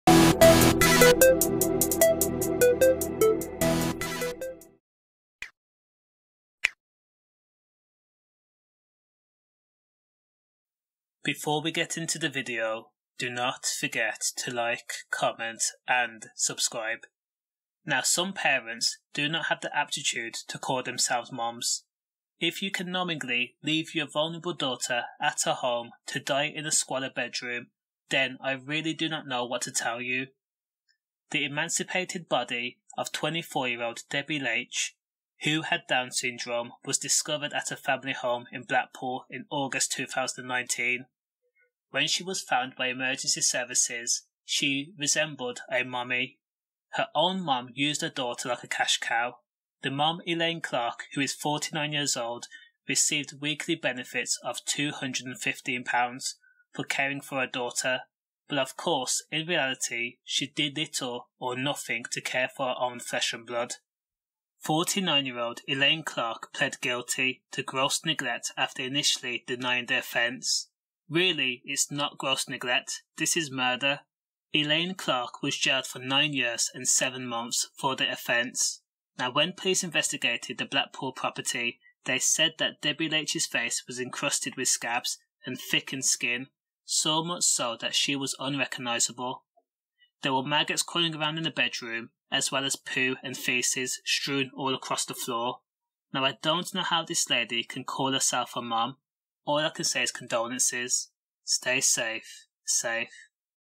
Mum Jailed For Starving Vulnerable Daughter To Death
A mum who slowly starved her own disabled daughter to death has been jailed for nine years and seven months.

Elaine Clarke, 49, confessed to killing her daughter and was convicted of gross negligent manslaughter of 24-year-old Debbie Leitch, who had Down’s Syndrome.

She died from severe emaciation and neglect - with the judge saying Clarke "abandoned her to die in pain, without nourishment in the most awful surroundings."

A post-mortem examination found that Debbie had an extensive scabies skin infection.

Her emaciated body was only discovered inside her house in Blackpool, Lancashire, by emergency services on August 29, 2019.

A court was told that she wasn't immediately "recognisable as a woman" as her face was crusted and there were maggots on the floor and the room was covered in dried faeces

Safe